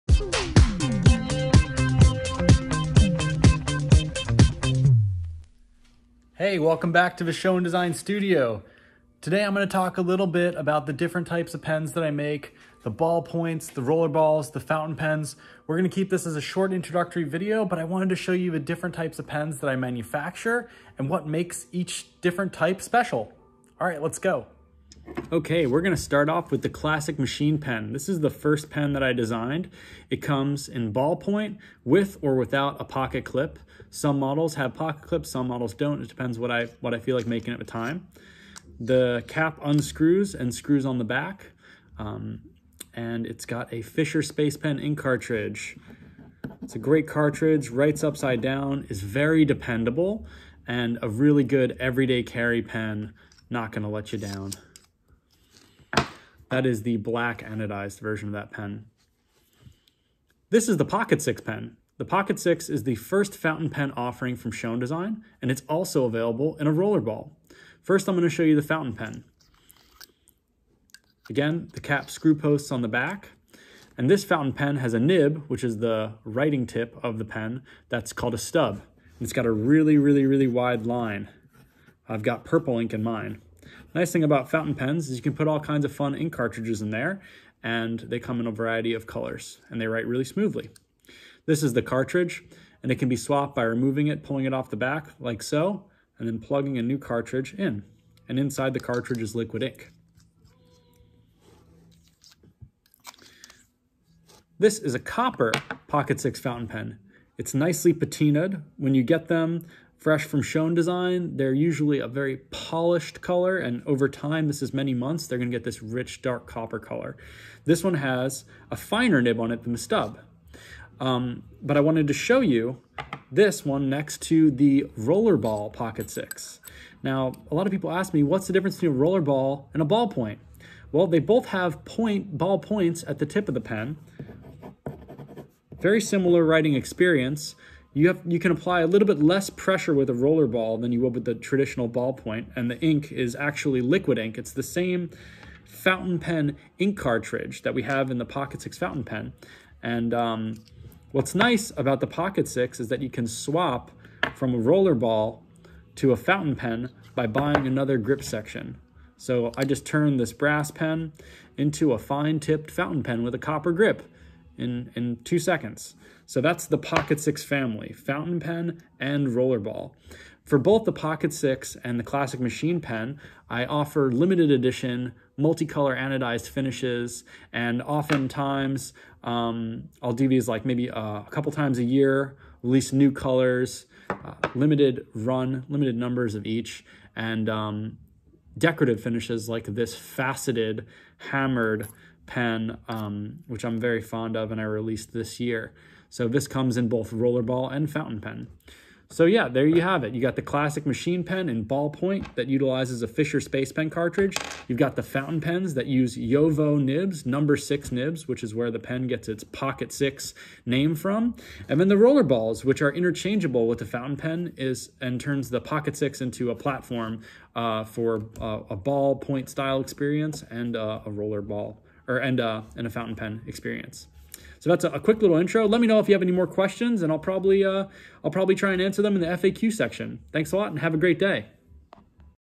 Schon DSGN Pens- 101! Classic Ballpoint, Fountain and Rollerball
A quick overview of the Classic Machined Pen, the Pocket Six fountain pen vs the Pocket Six Rollerball pen!
Big question is whats the difference between the Ballpoint and Rollerball?! Well, the rollerball uses liquid fountain pen ink, but acts like the ballpoint (which uses a fisher space pen cartridge). The liquid ink in the rollerball writes very smootly , just like a fountain pen and allows for the use of many colors of fountain pen ink! Figured I make a quick video to show how these compare